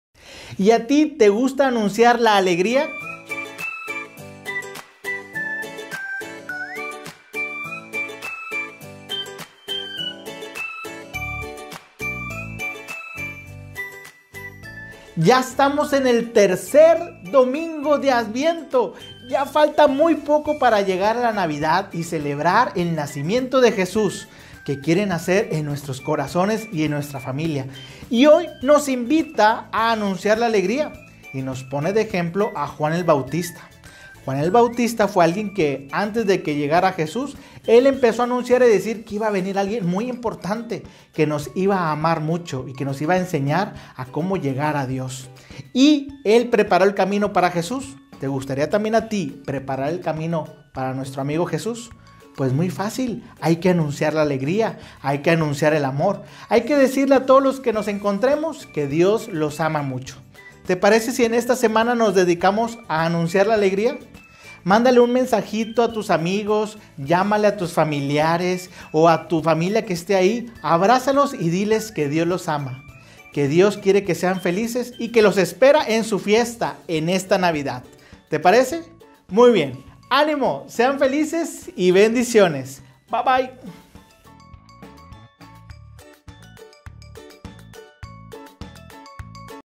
El Evangelio para niños
Y a ti, ¿te gusta anunciar la alegría?

El Evangelio para niños, con el Padre Borre.

Te esperamos todos los domingos a las 10:00am en la transmisión de la Misa con niños, aquí en nuestro canal 😊😇